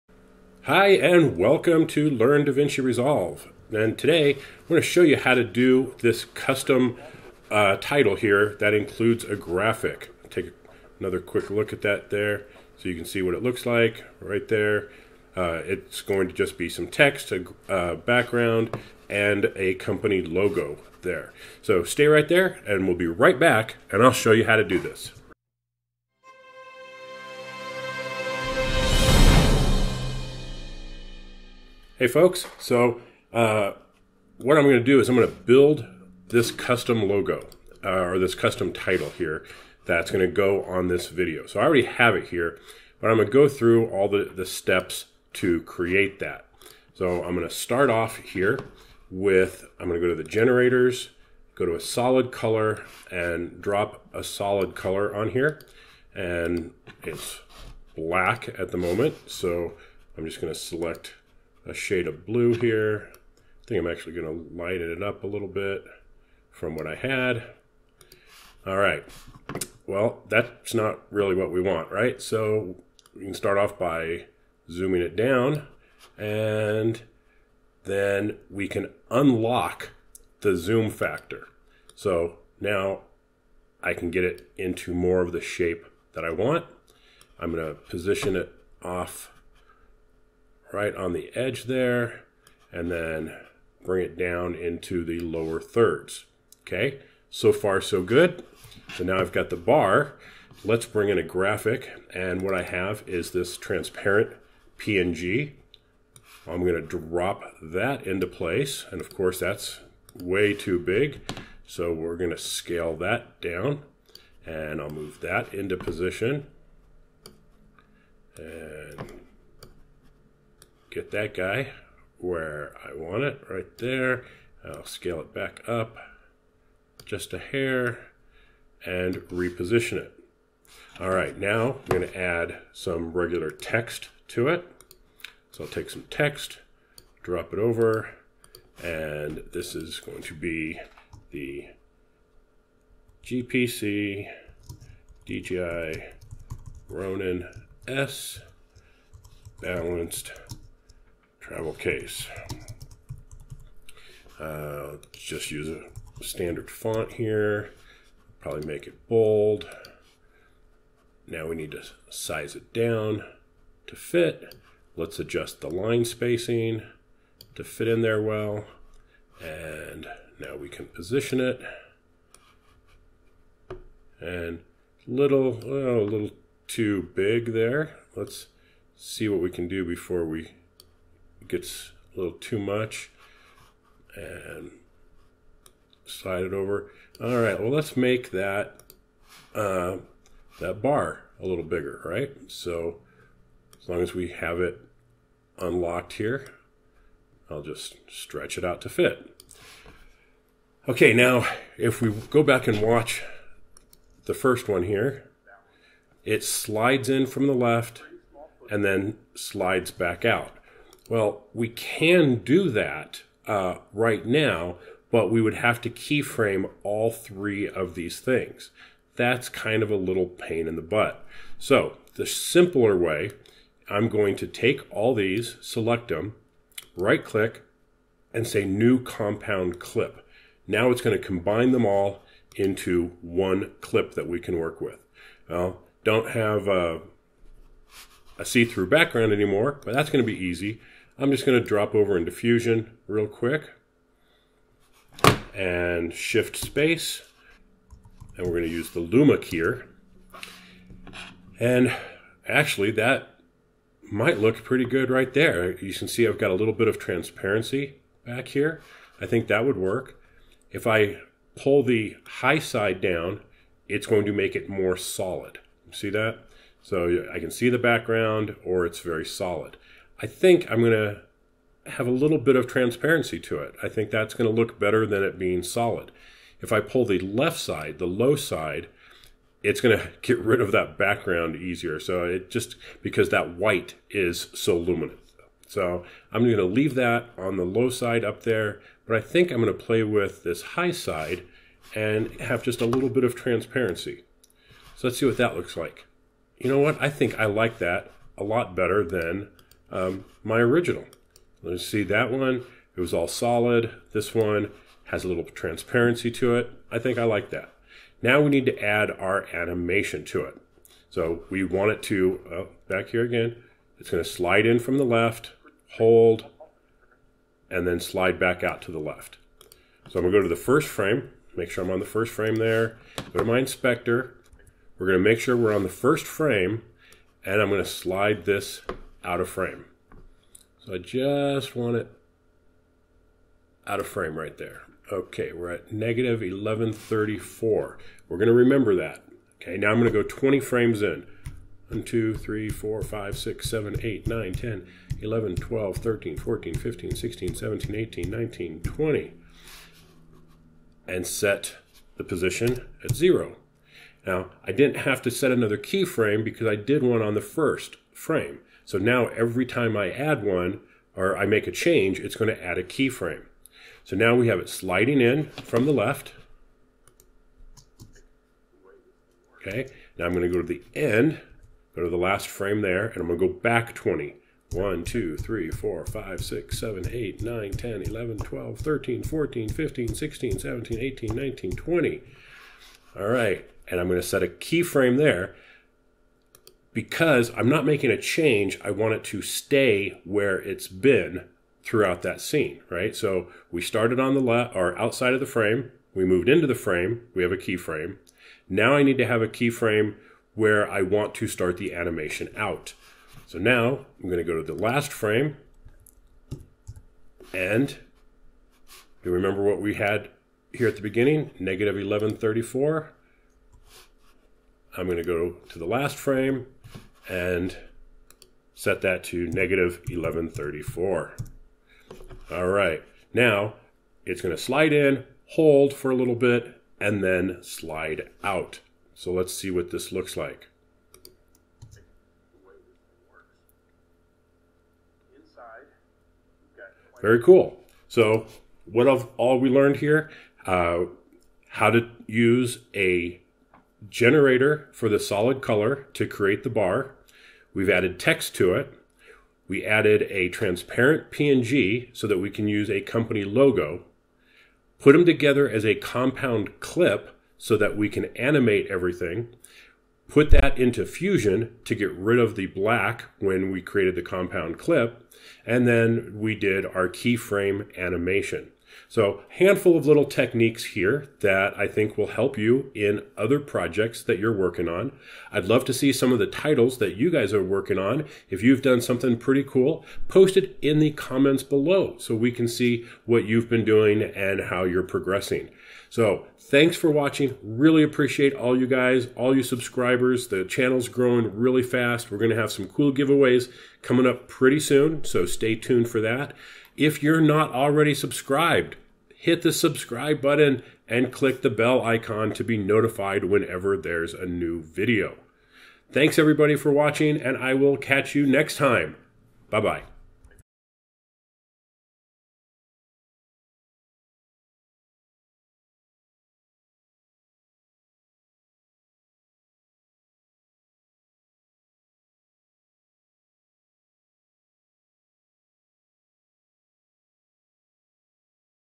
Custom animated lower thirds - Davinci Resolve Fusion
Today I show you how to make a custom animated title bar that incorporates a logo and text.

Definitive Guide to Davinci Resolve 15